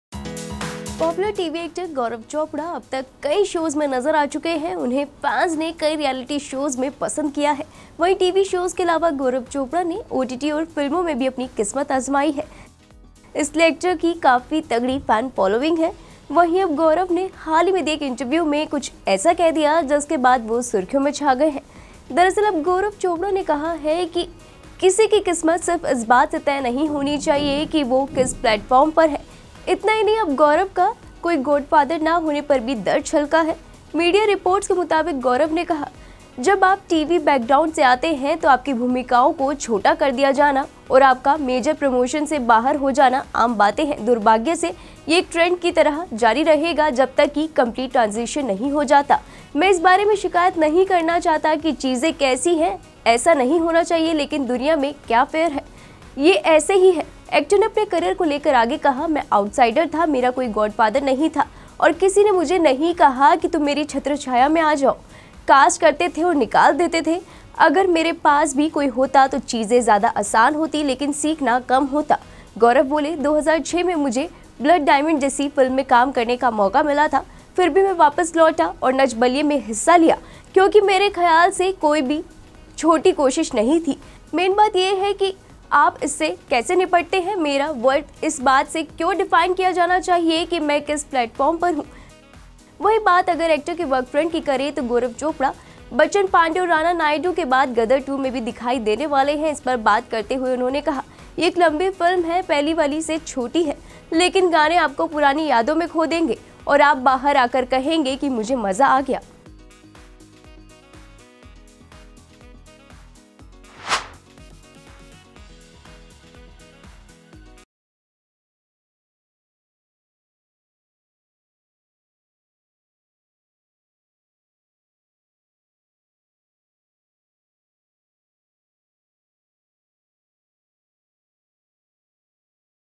Gaurav Chopra को बॉलीवुड में नहीं मिली कोई खास पहचान, अब सालों बाद छलका एक्टर का दर्द
Now Gaurav said something like this in a recent interview, after which he is in headlines. In fact, now Gaurav Chopra has said that someone's worth should not be decided just by the platform he is on. Not only this, now there is pain even after Gaurav does not have any godfather.